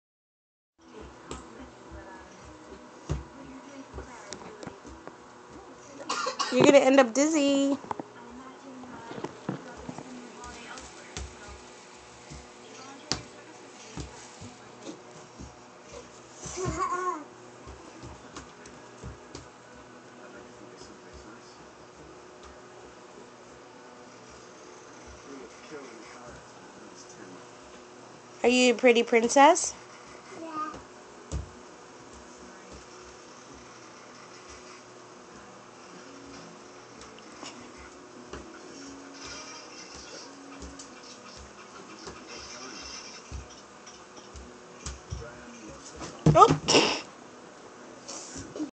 Kim the ballerina princess
Just caught Kim spinning in circles before she falls.